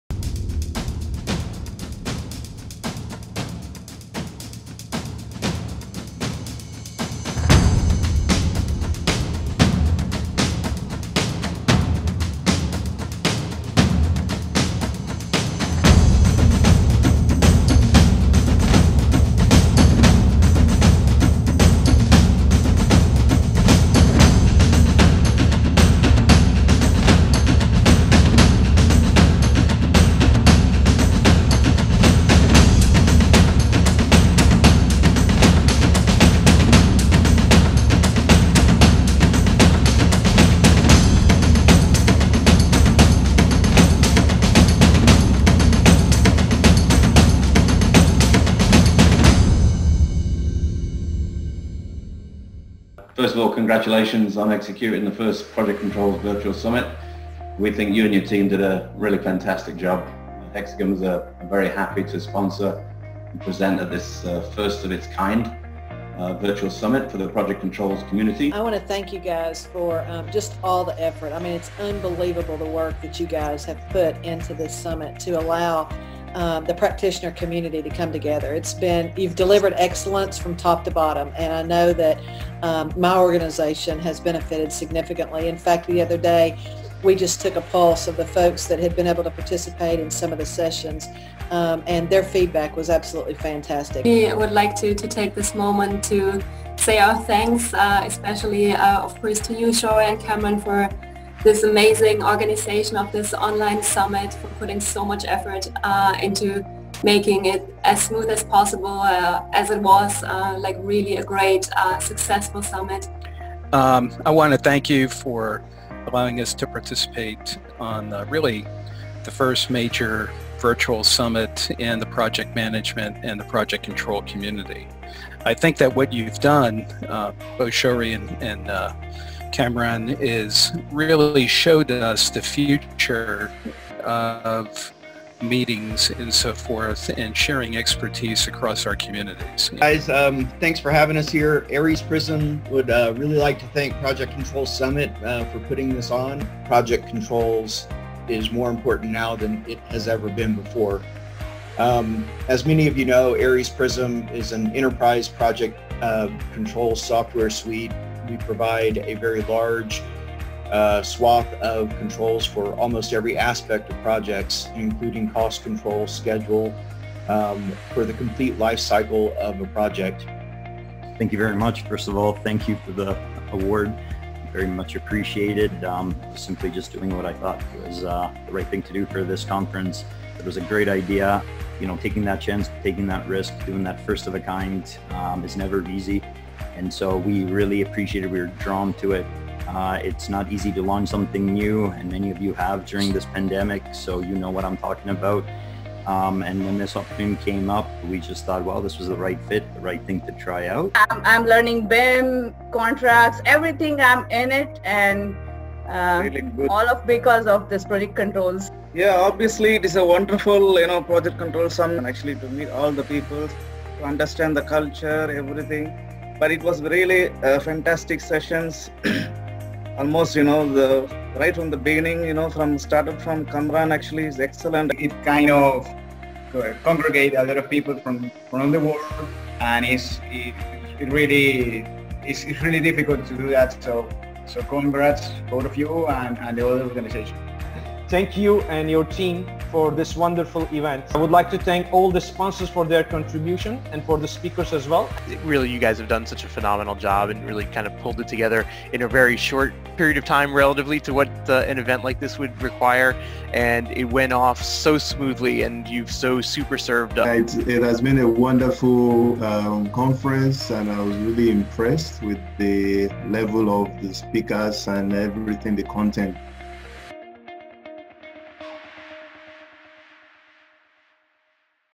2020 Project Control Summit Feedback
In 2020, we launched the first-ever Global Virtual Conference in the Project Controls space, which was well-received by the global audience. We welcomed over a thousand project professionals from 70+ countries and all continents. Check this video for the feedback received on the 2020 Project Control Summit.

To get more information about this year's conference and to get your ticket, please

Video Chapters:

00:00- Summit Intro
0:57- Feedback from Mark Nelson, Hexagon
1:08- Feedback from Misty Mayes, Management Solutions
1:34- Feedback from Vanessa Sutterlüti, Cost Engineering
1:53- Feedback from Nick Pisano, SNA Software
2:19- Feedback from Russ Stevens, ARES Prism
2:50- Feedback from Kris Stozek, Modus Services
3:20- Feedback from Ishita Shah
3:28- Feedback from Dayanidhi Dhandapany
3:45- Feedback from Diego Nicolas
3:57- Feedback from Abdullah AKPINAR
4:05- Feeback from Adam Goldfarb, Hexagon